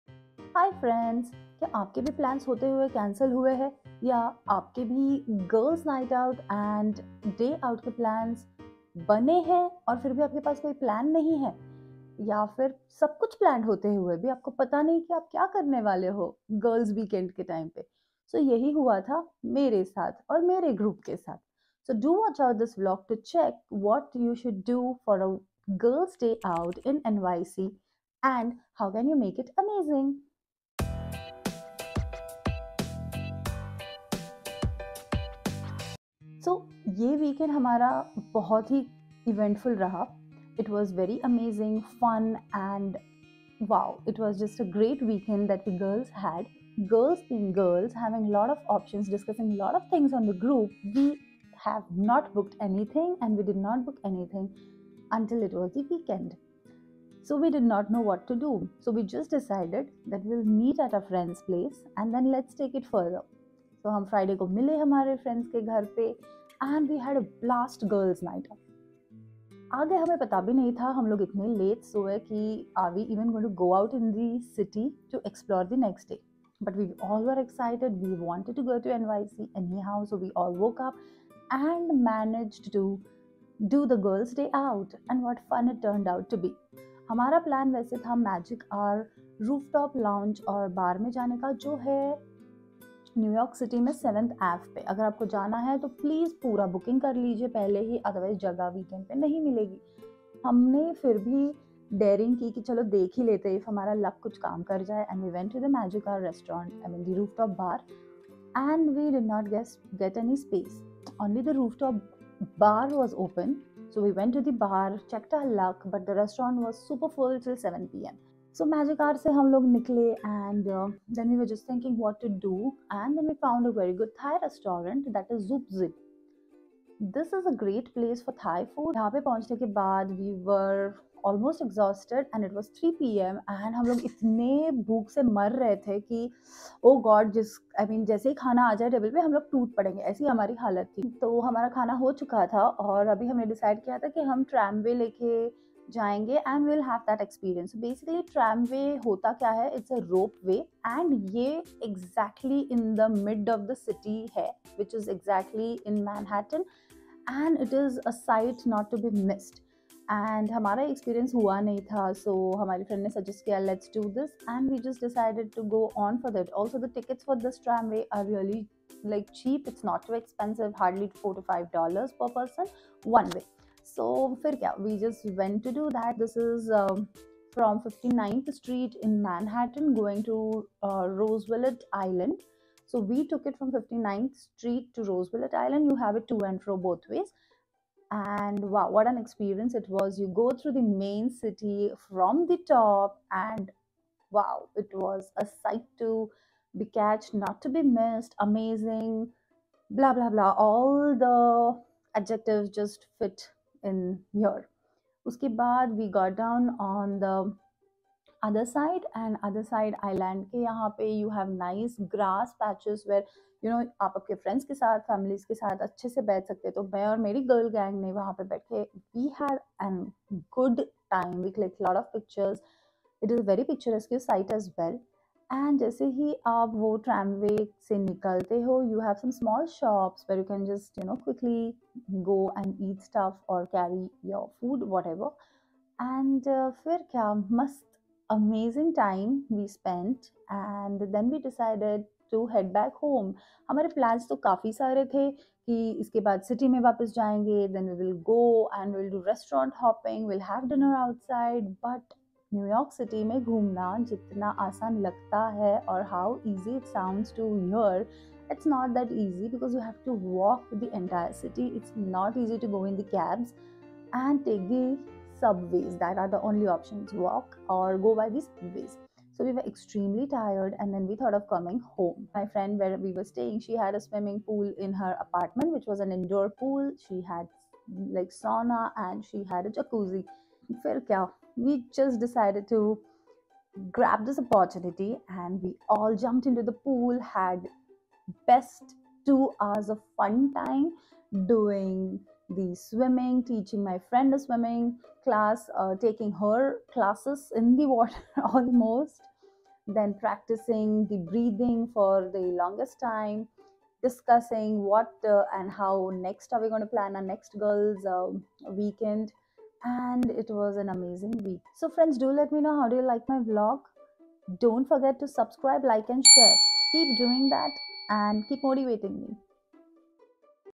Girls Day Out | Roosevelt Island Tramway| New York City | Things You Can Do
Things to do on a day out in NYC

Magic hour Roof top bar & lounge

Zoob zib authentic Thai restaurant

Roosevelt Island Tramway